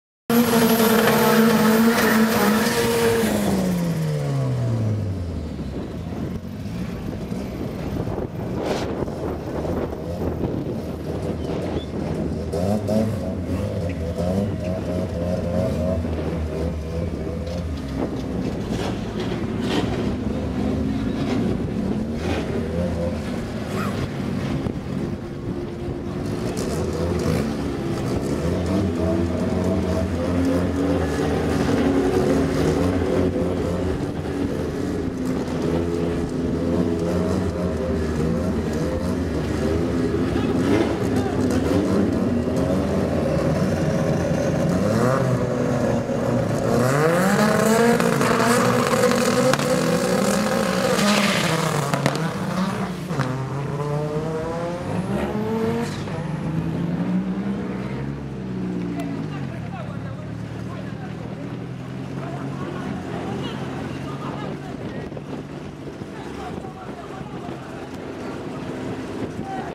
May 2014 QTM Day 3 Run 46
04th May 2014 - Day 3 "Eliminations and Finals" of the 2nd Quarter Mile Event for 2014, Hosted by the MDRA at Hal Far Raceway.